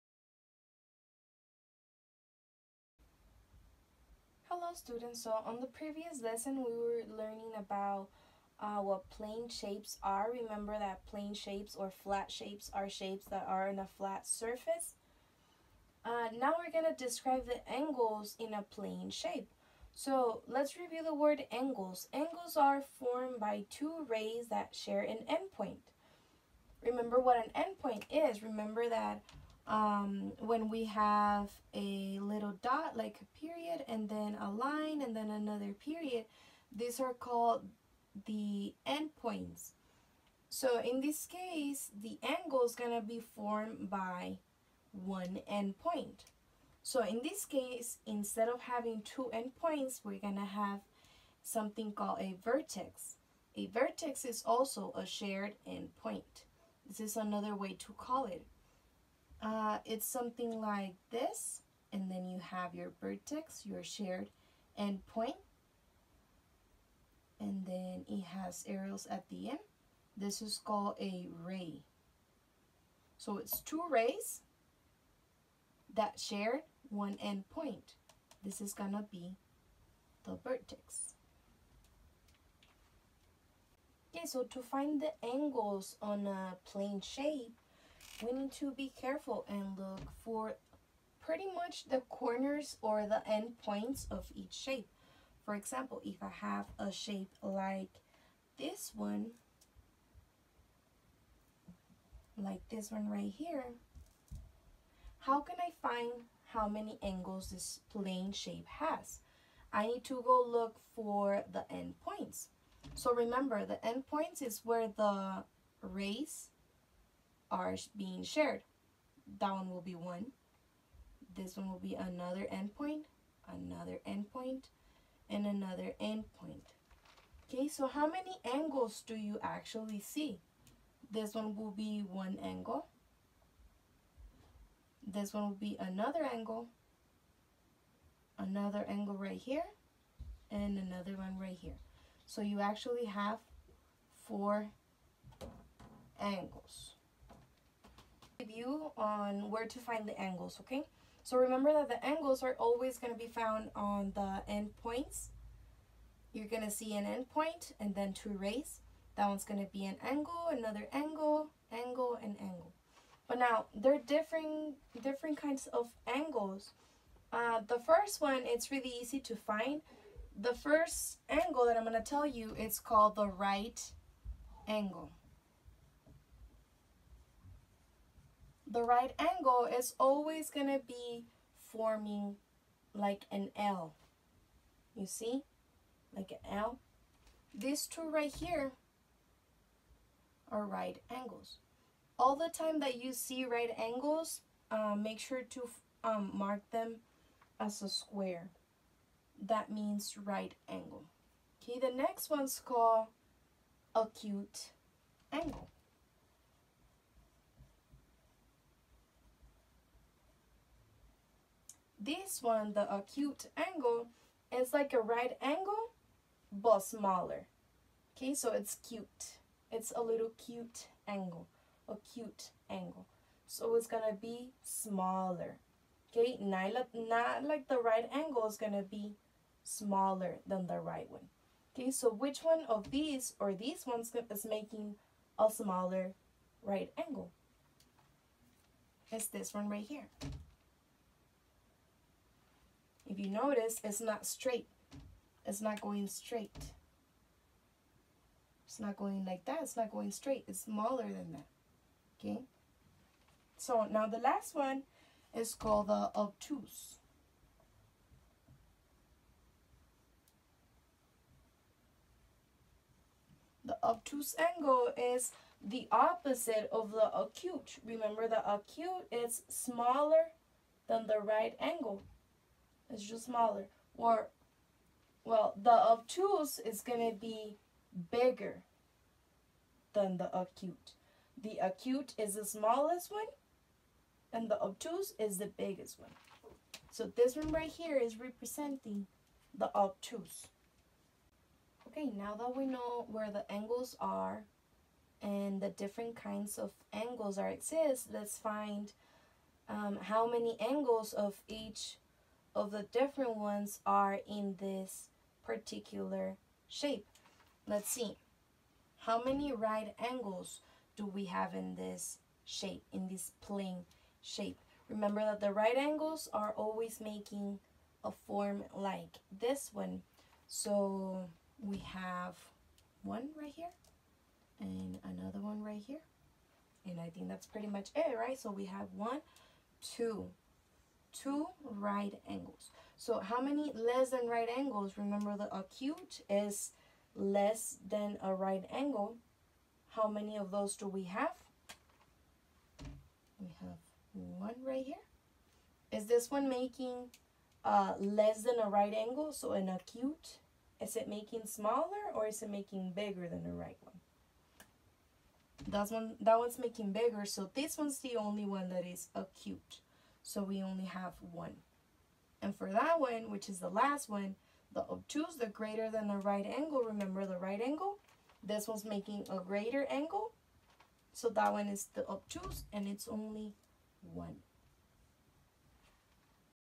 3rd grade math lesson 12.2 Describe angles in plane shapes
Hello!!
For his video I will be explaining what angles are, how to identify them and the different kinds of angles that you can find on a plane shape!!